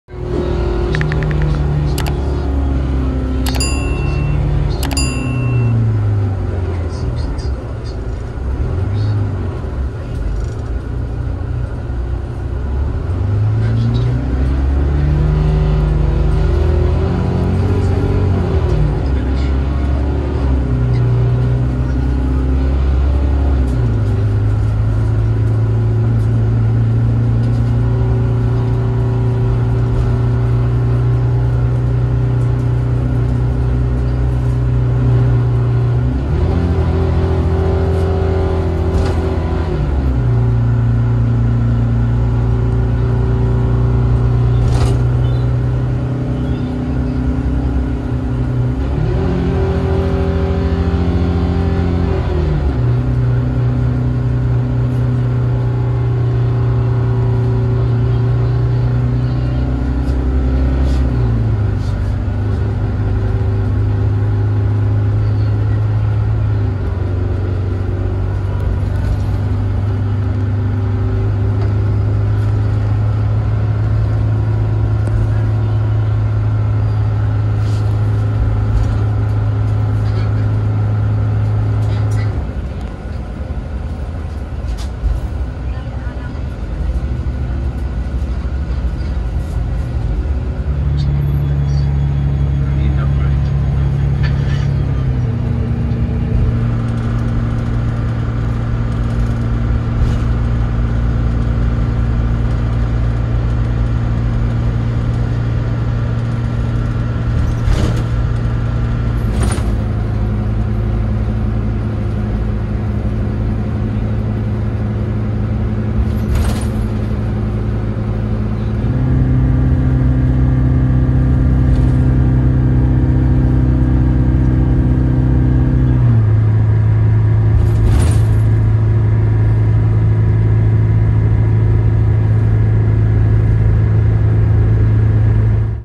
330hp Volvo B8R Bus *PURE SOUND* | ASMR
⚠️WARNING: HIGHLY ADDICTIVE ⚠️

Di ka pa nakakasakay ng Volvo Bus at curious ka kung anong tunog sa loob? PUT ON YOUR HEADPHONES and watch this video para malaman!😍